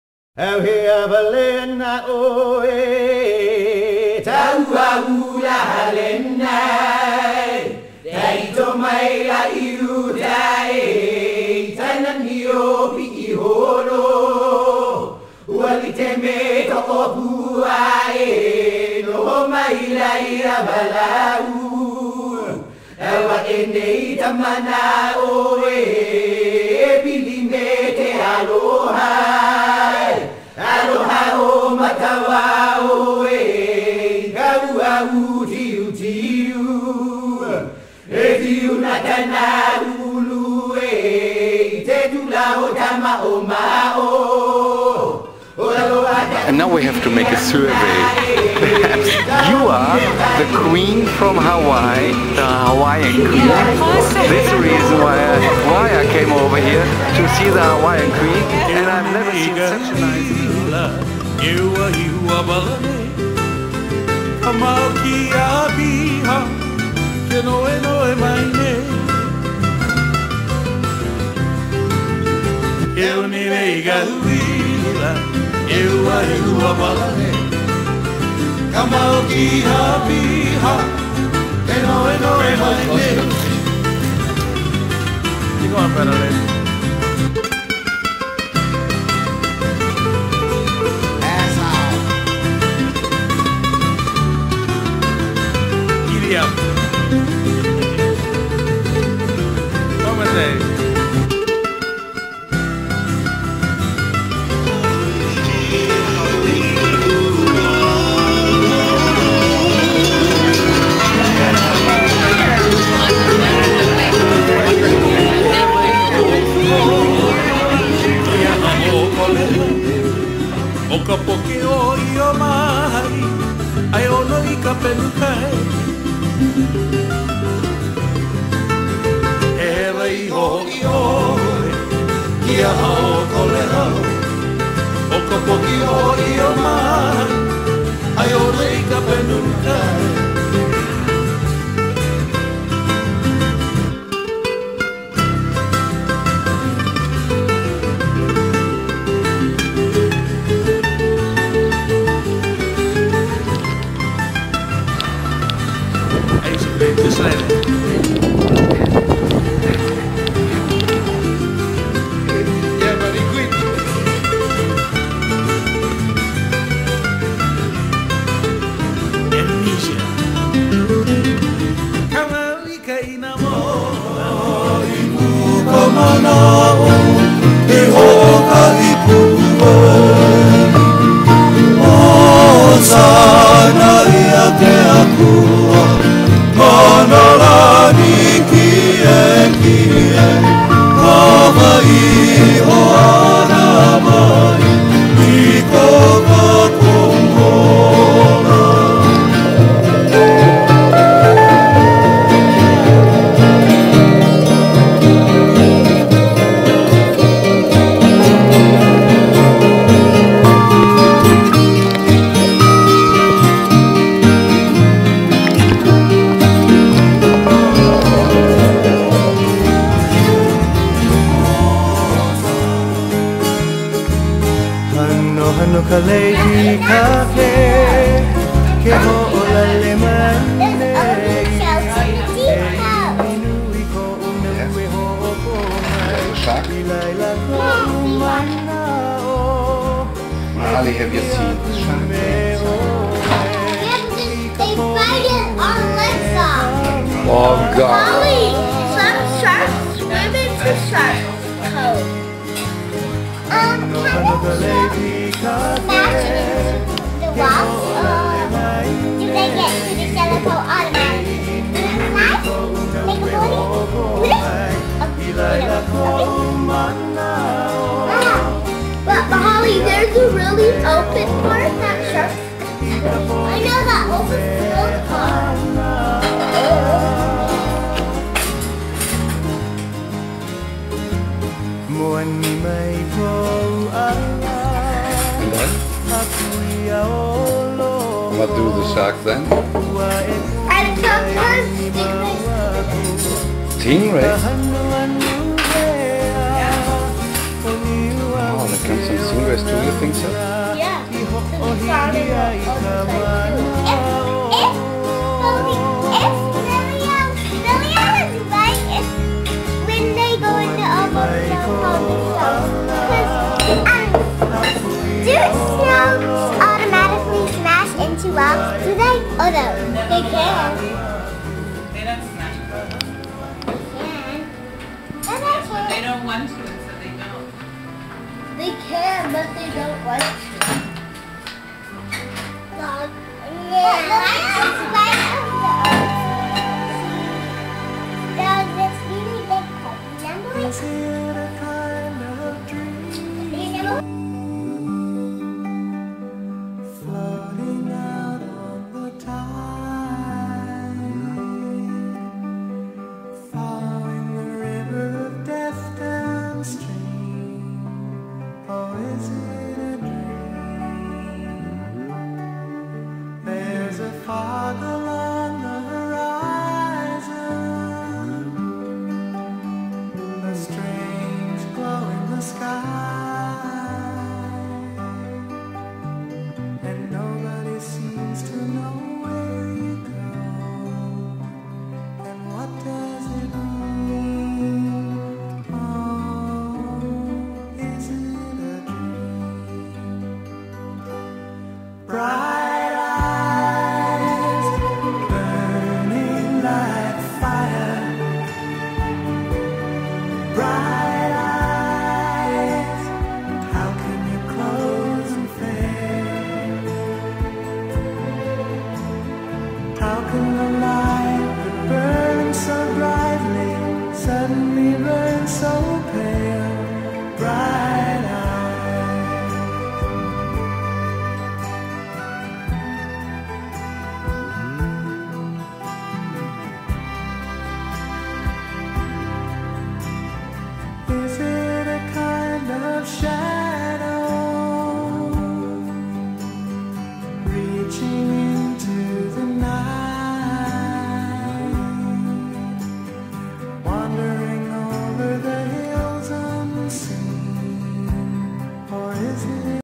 Hawai'i Luau Peanut July 2009
Luau Party from Peanut in Hawai'i
Dzung & Hajo Visit O'ahu